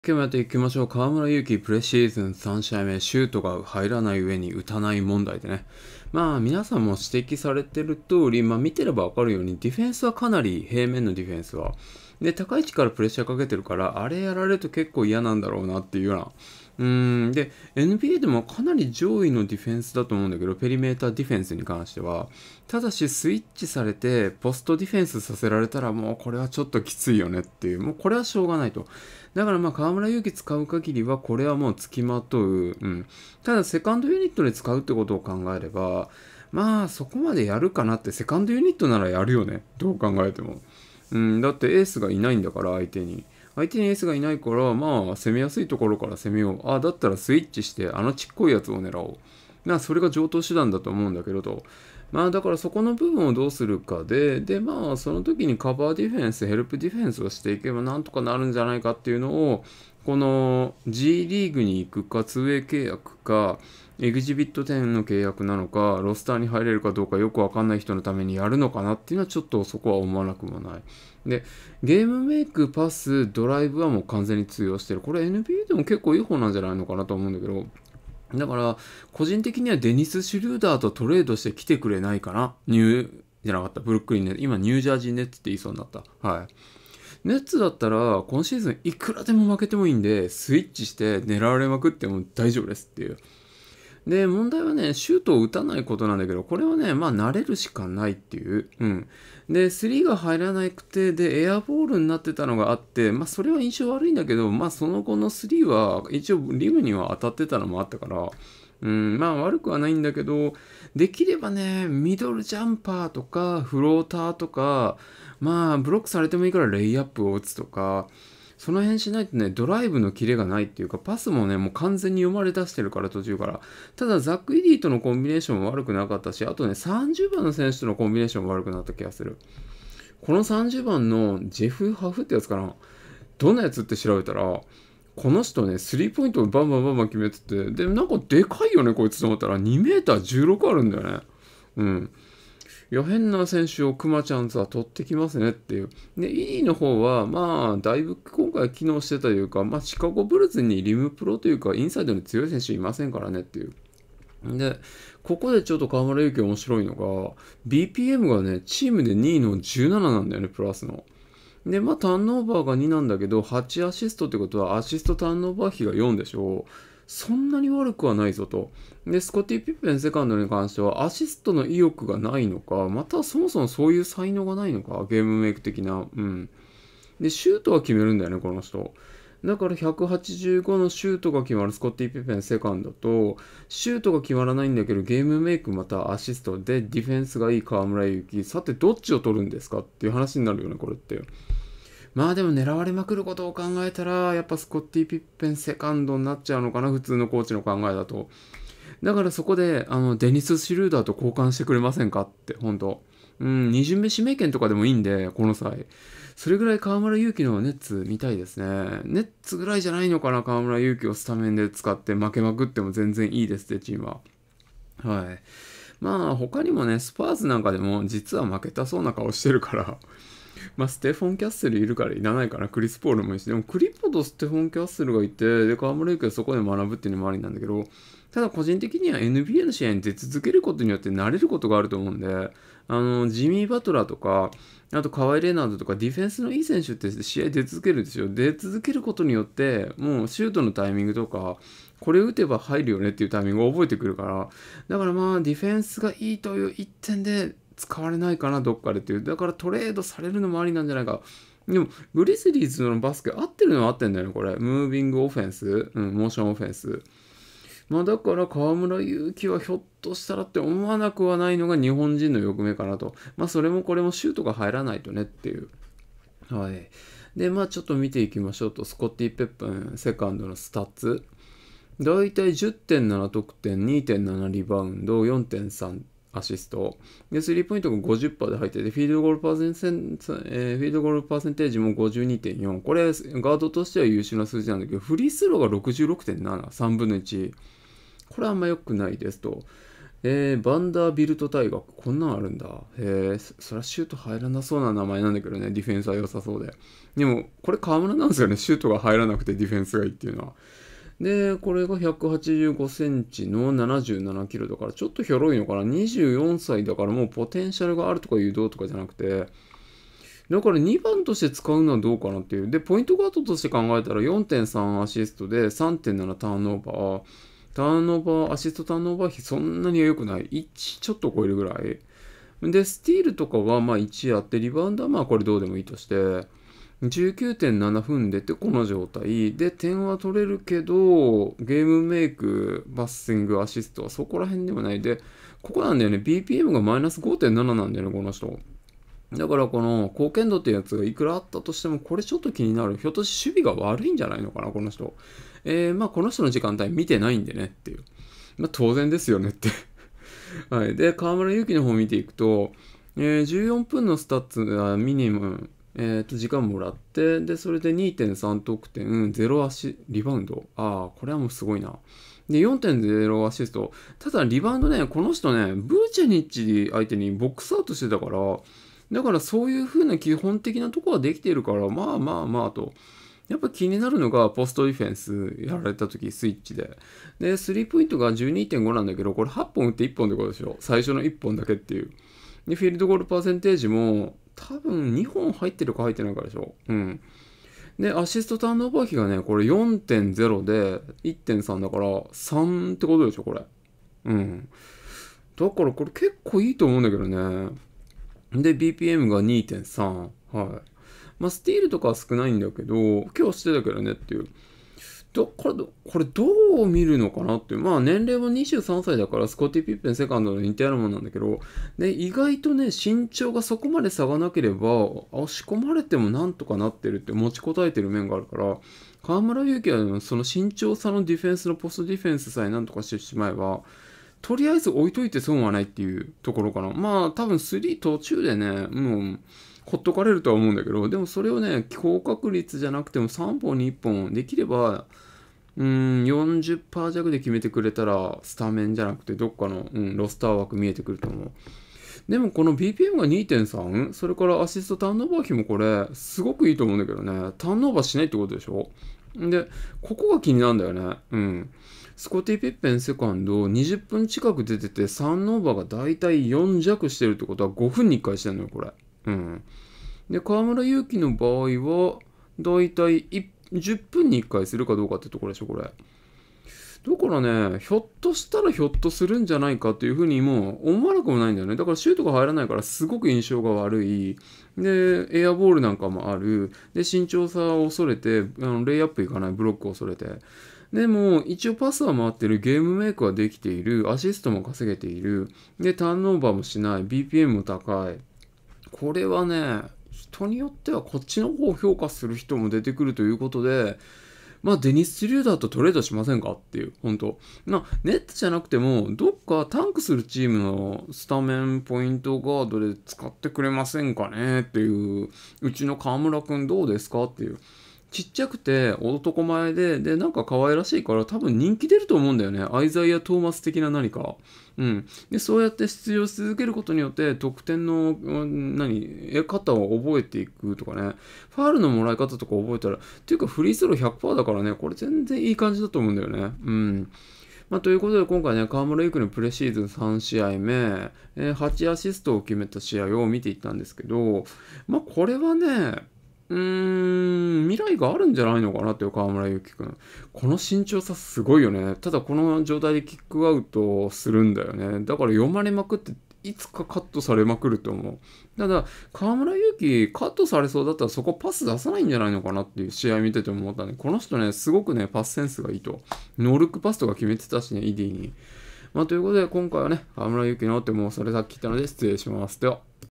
河村勇輝プレシーズン３試合目「シュートが入らない上に打たない問題はあるが、いいところも大いにある」
以下のチャンネルもよろ🫠よろ

万が一、BANされた時用の候補地です